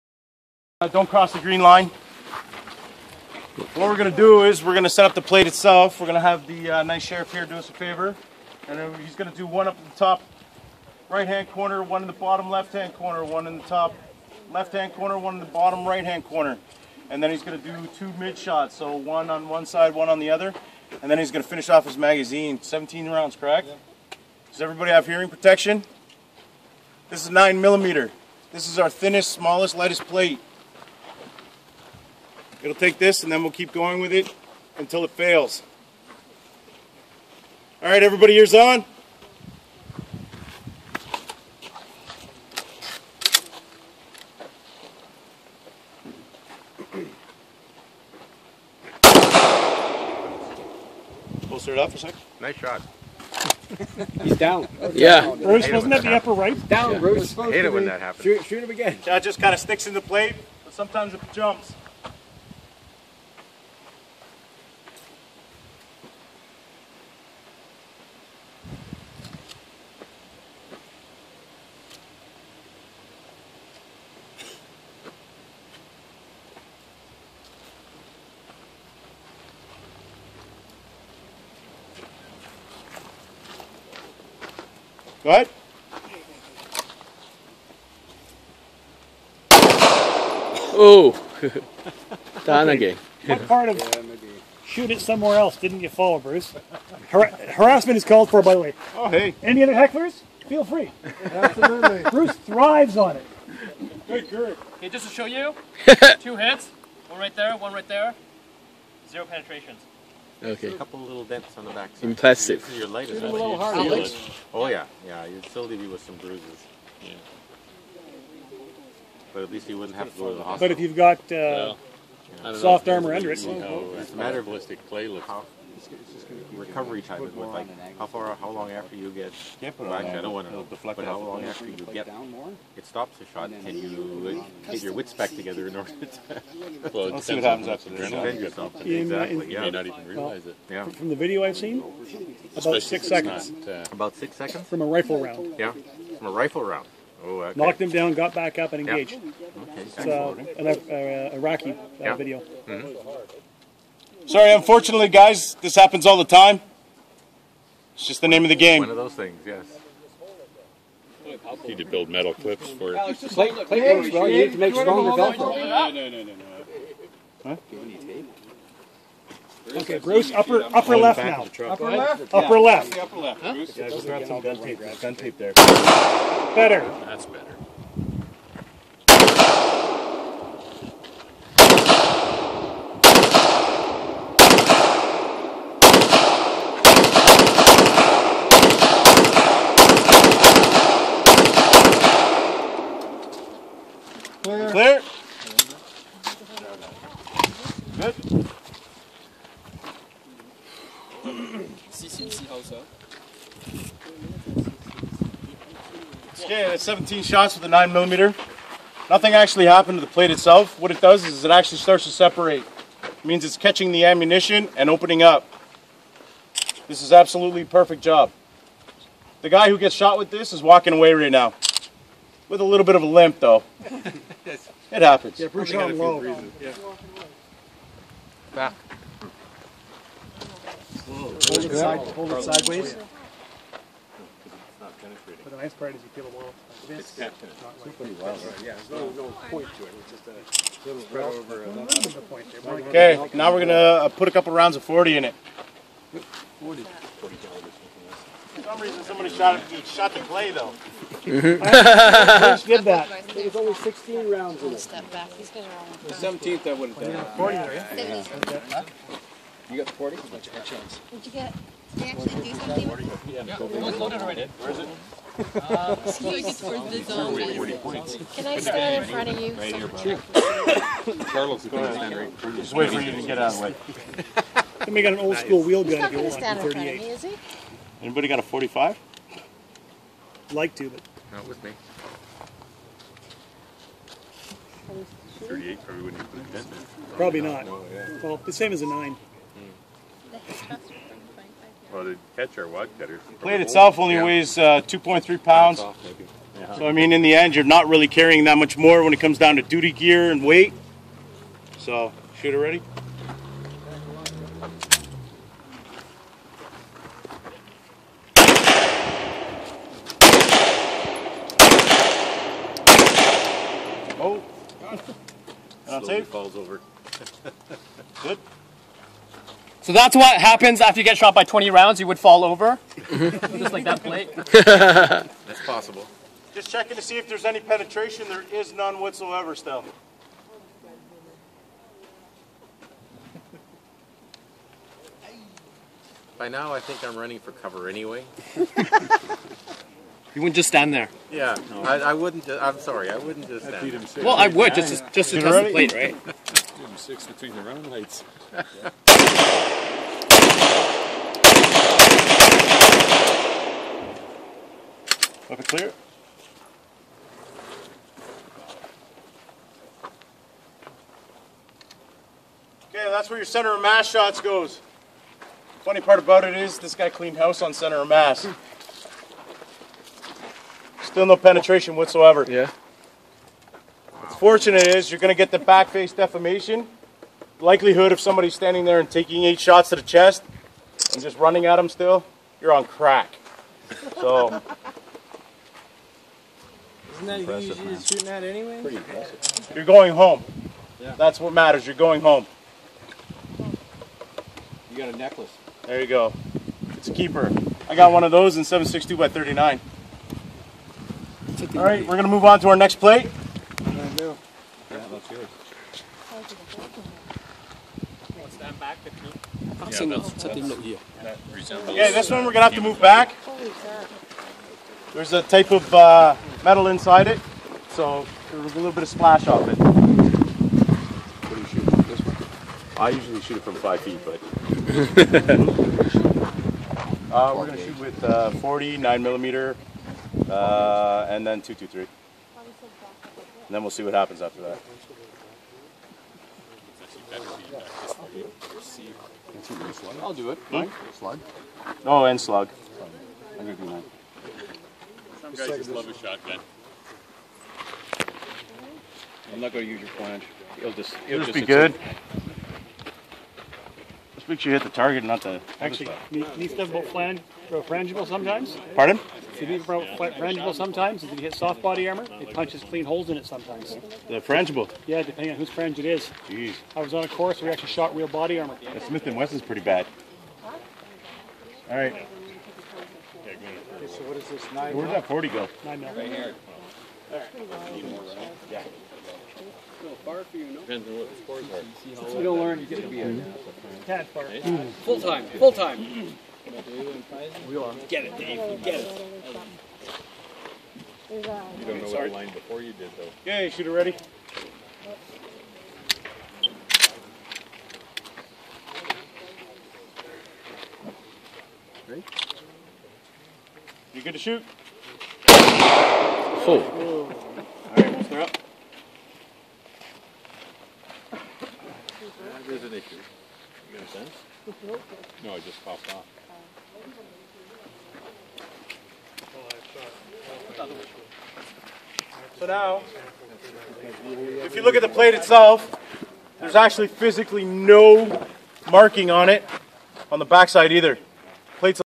Sterling Cross Canadian Body Armour Testing by shooting 9mm & 40 pistol
GunsTalkChinese recorded the Sterling Cross Body Armour Plate Testing by 9mm and 40 pistol
Videographer: Tim Fan